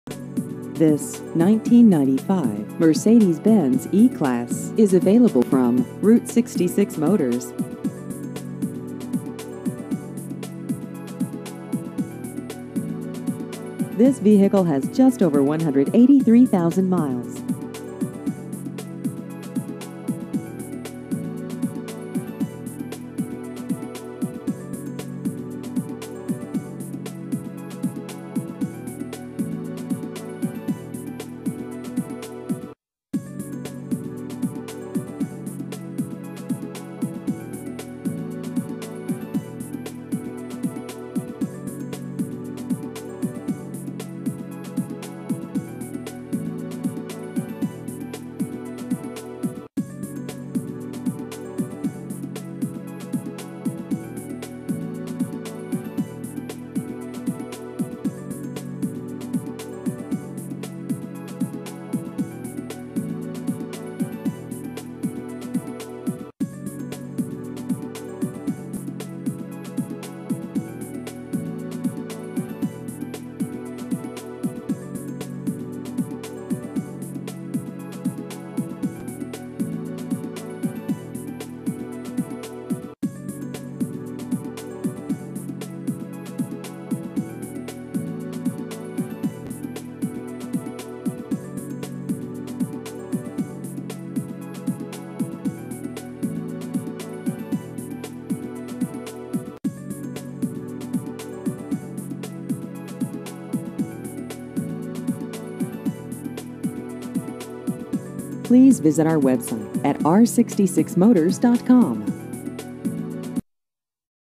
1995 Mercedes-Benz E-Class Used Cars Baldwin Park or Glendor
This 1995 Mercedes-Benz E-Class is available from Route 66 Motors.